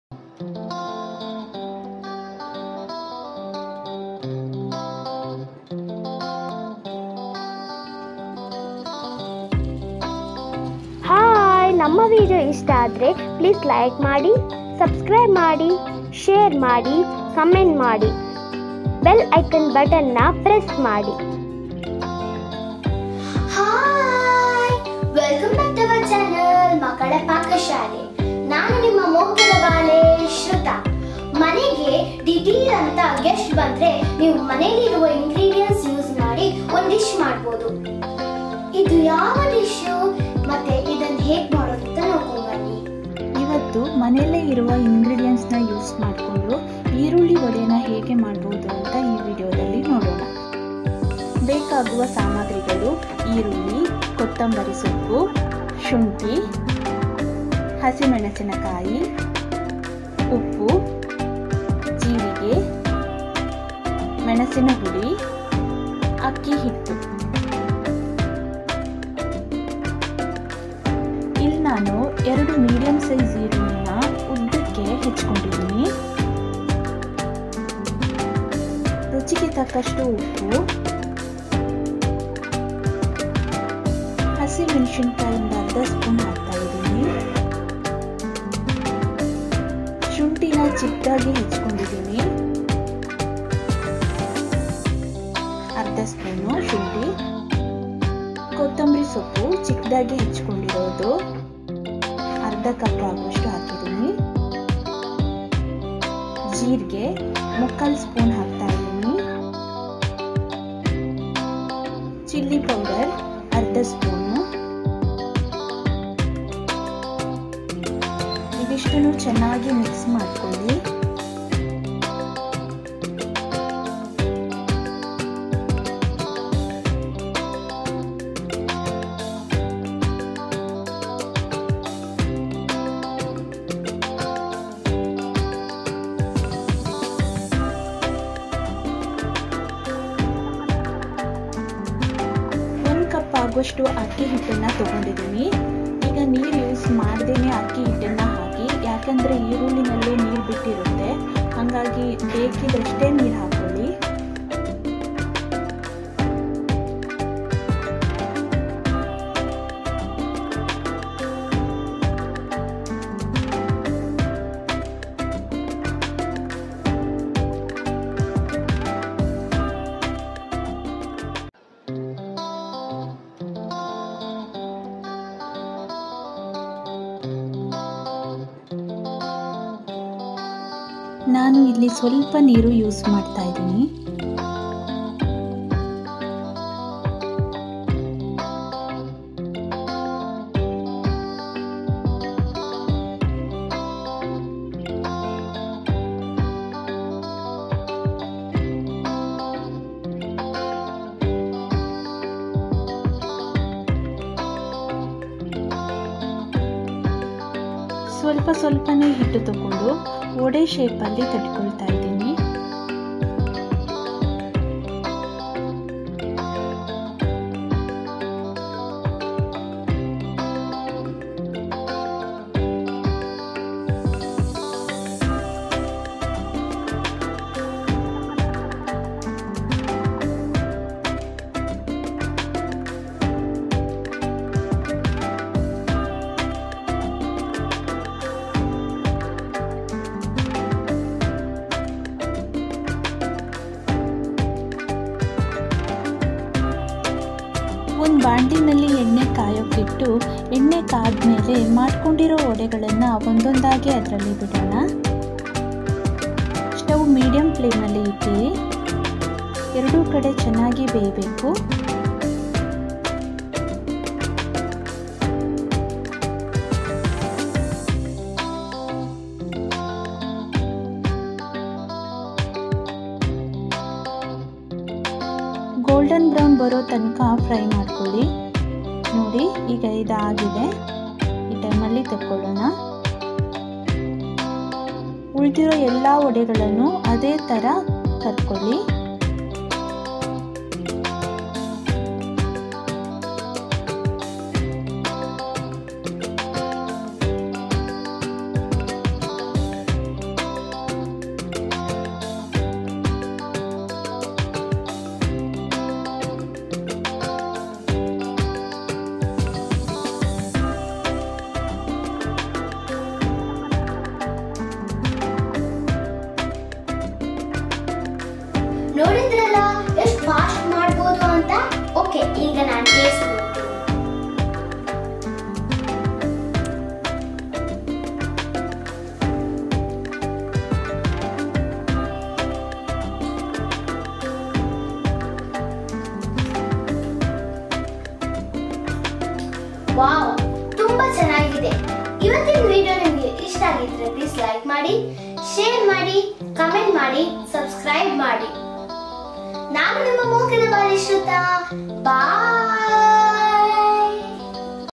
ಗರಿ ಗರಿಯಾದ ಈರುಳ್ಳಿ ವಡೆ ಮಾಡುವುದು ತುಂಬಾ ಸುಲಭ |Easy & quick tea-time snacks onion vada | evening snack
ಗರಿ ಗರಿಯಾದ ಈರುಳ್ಳಿ ವಡೆ ತಯಾರಿಸುವ ವಿಧಾನ
Easy & quick onion vada preparation.
Video by Makkala Paakashaale (ಮಕ್ಕಳ ಪಾಕಶಾಲೆ)